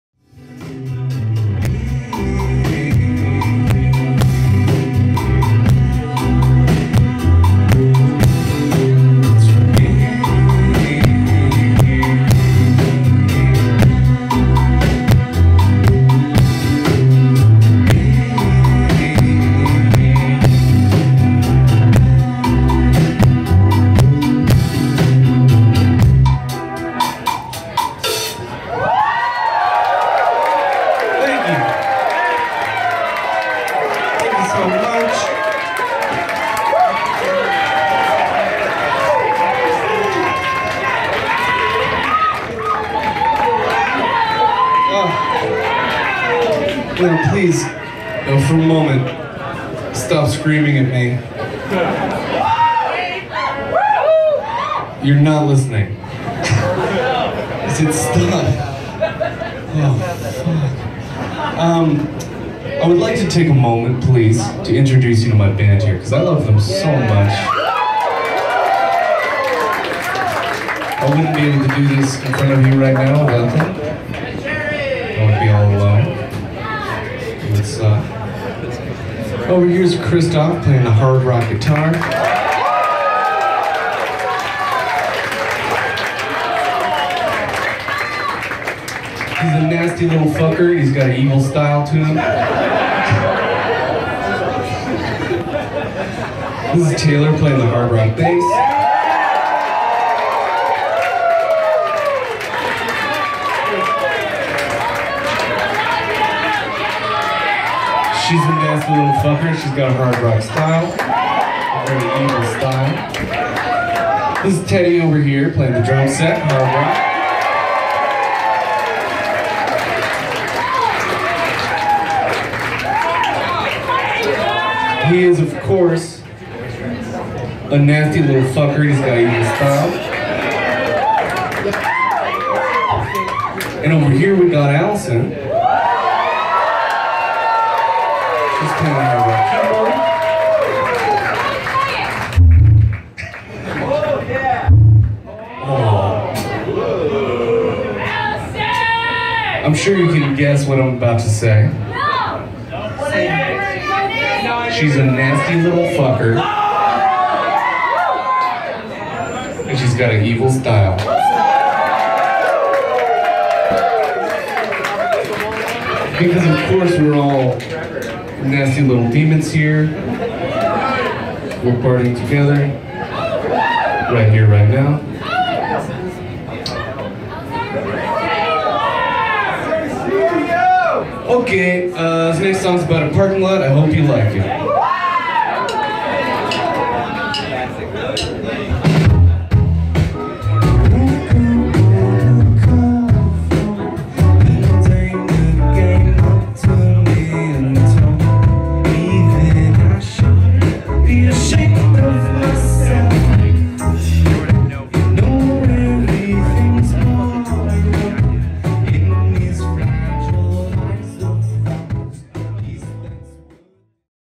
Jerry Paper Introduces the Band
Jerry Paper live in Chicago, IL @ The Empty Bottle